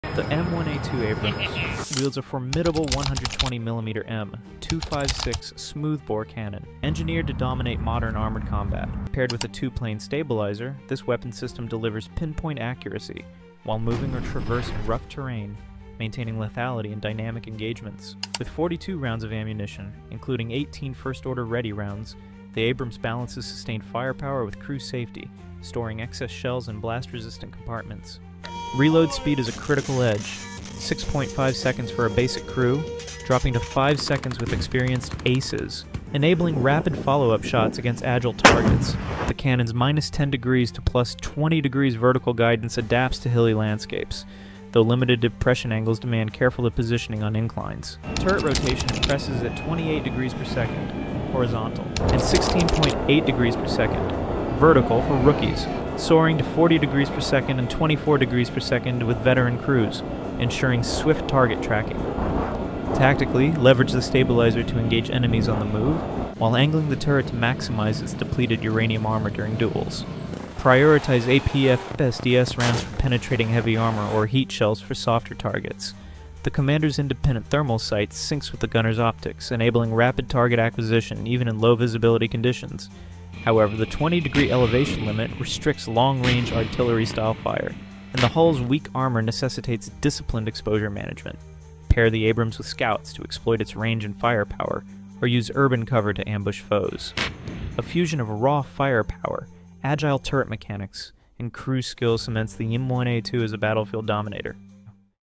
M1A2 Abrams Firepower: 120 mm M256 Cannon, Reload Speed & Turret Tactics Explained
M1A2 Abrams Firepower: 120 mm M256 Cannon, Reload Speed & Turret Tactics Explained | Armored Combat Breakdown

Unlock the secrets of the M1A2 Abrams’ lethal 120 mm M256 smoothbore cannon—the backbone of modern armored warfare. Engineered with a two-plane stabilizer, this cannon delivers pinpoint accuracy on the move, even over rough terrain. Discover how 6.5-second reloads (dropping to 5 seconds with veteran crews) and turret rotation speeds of 40°/s turn the Abrams into a rapid-response juggernaut.
Tactically, leverage APFSDS rounds to shred heavy armor or HEAT shells for infantry and soft targets. The commander’s independent thermal sight ensures rapid target acquisition in smoke, dust, or darkness. But beware: the 20° elevation limit restricts indirect fire, and the hull’s weak armor demands smart positioning. Learn how to maximize depleted uranium turret armor, pair with scouts, and exploit urban ambush zones. A masterclass in firepower, speed, and crew skill—is this the ultimate armored combat machine?